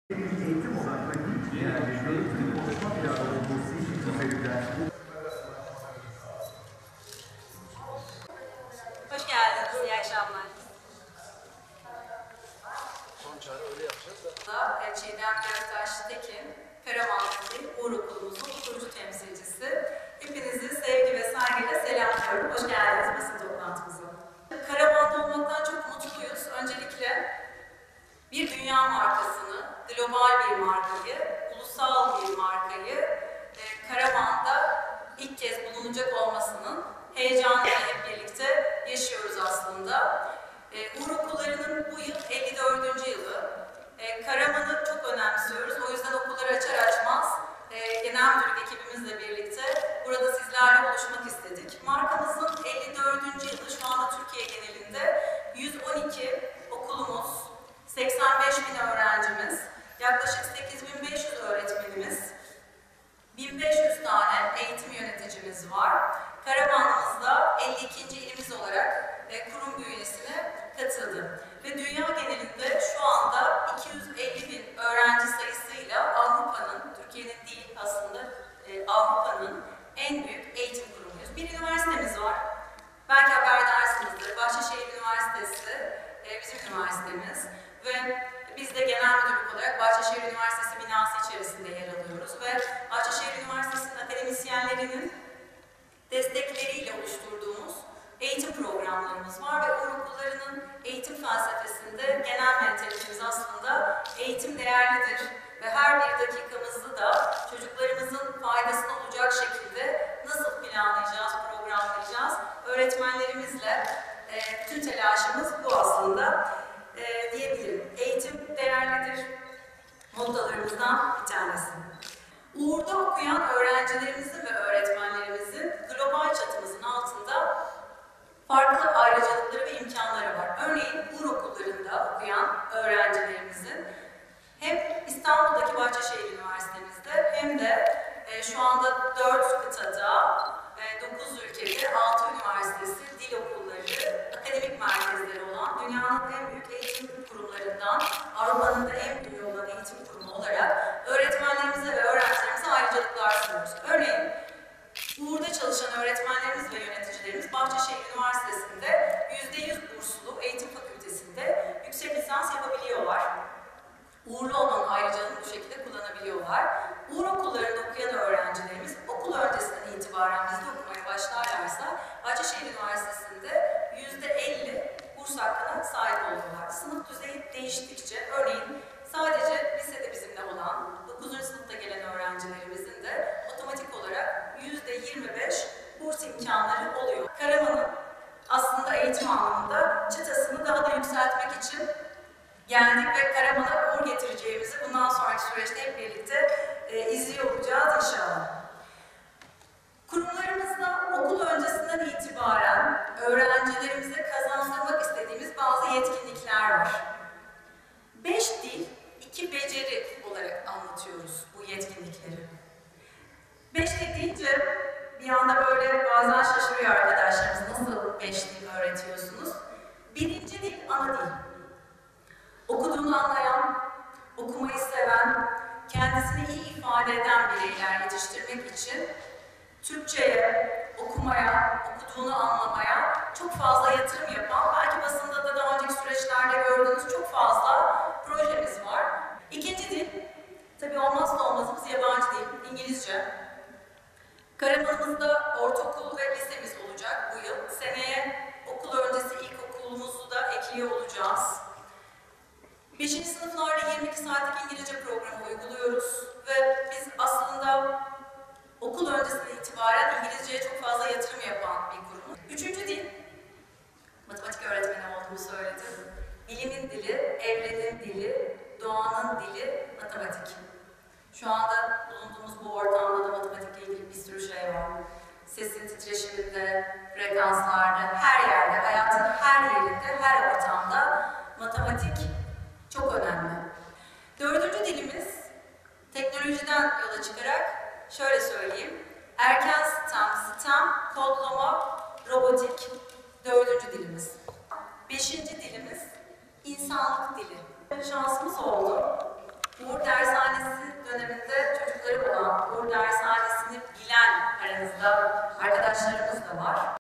Karaman'a Uğur geldi
54 yıllık deneyim, vizyon ve uluslararası başarısıyla eğitim-öğretime devam eden Uğur Okulları, Karaman Kampüsünde öğrencilerle buluşuyor.